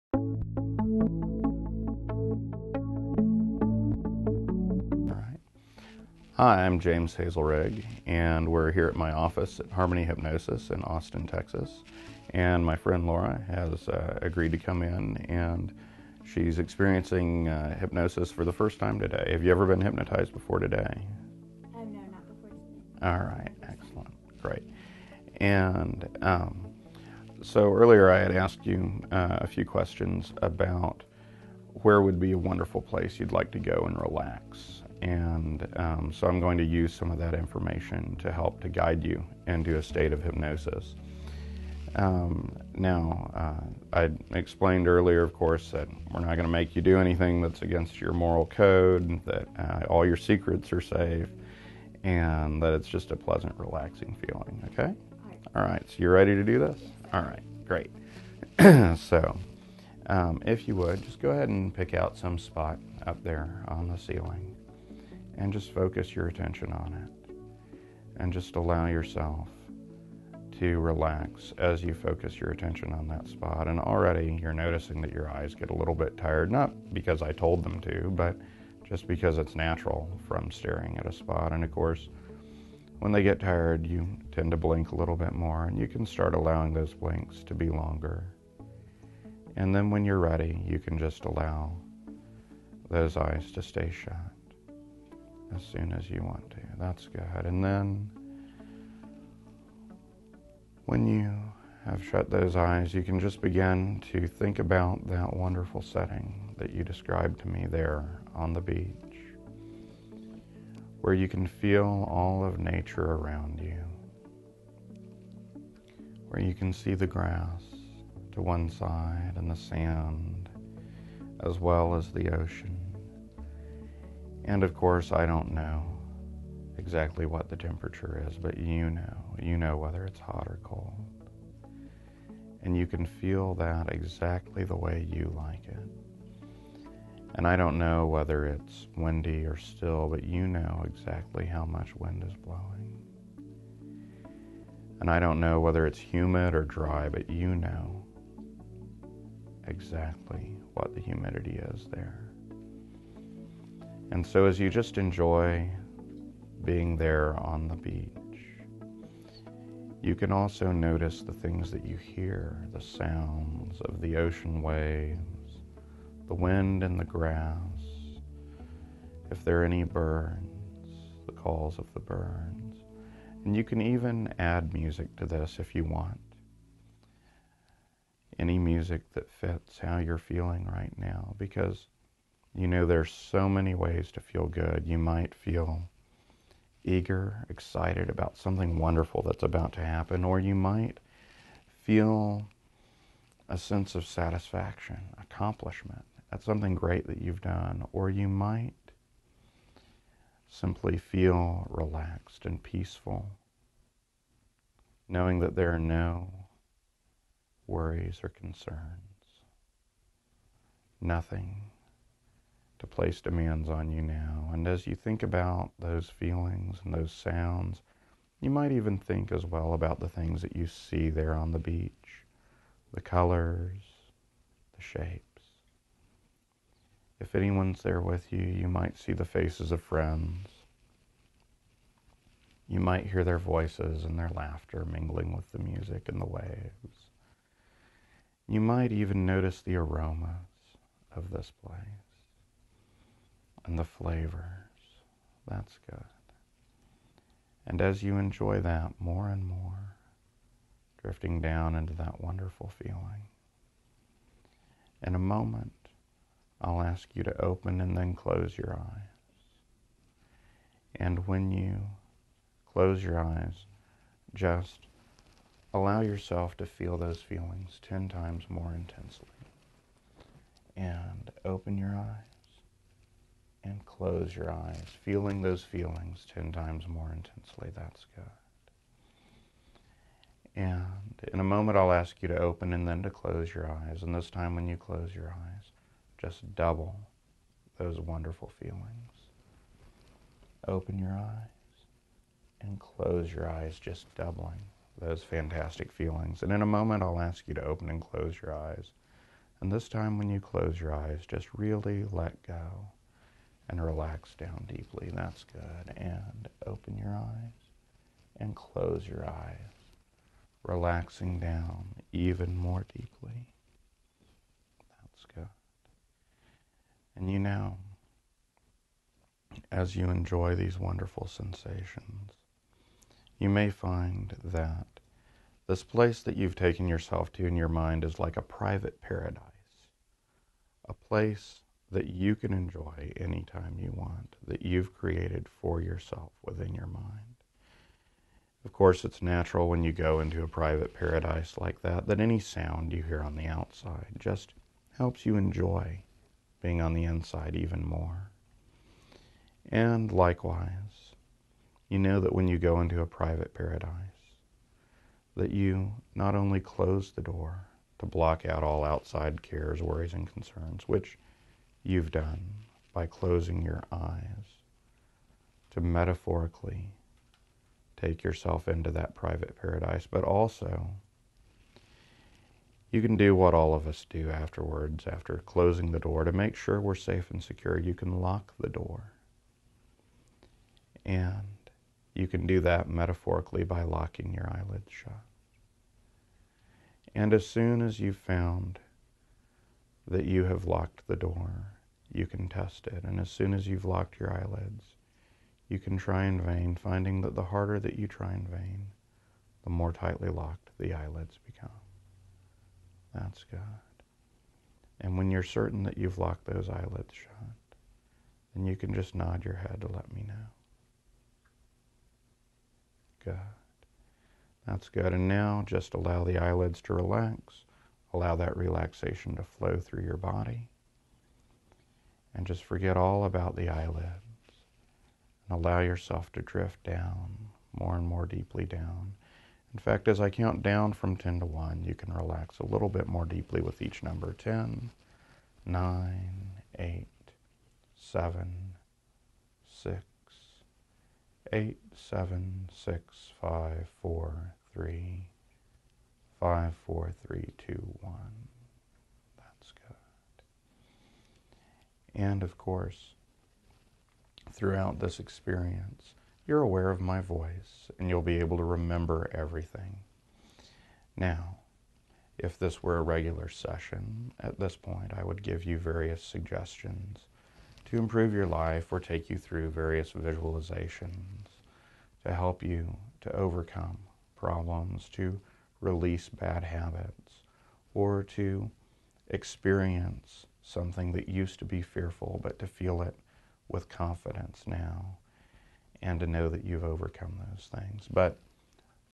What a Hypnosis Session Looks Like--Part One
Hypnosis is exciting, but the exciting parts happen within the trancer's mind. Laura was kind enough to let me guide her through a gentle, enjoyable hypnotic process of hypnosis.

While this is not as flashy as the instant inductions that so many videos feature, it is much more enjoyable for the trancer. After all, the point of a hypnosis session is not to look good on YouTube, but to facilitate the changes the client needs.

From this simple beginning, you could visit my very safe, comforting office in Austin, Texas, to use hypnosis to:

or live free from fears, even if they're not full-blown phobias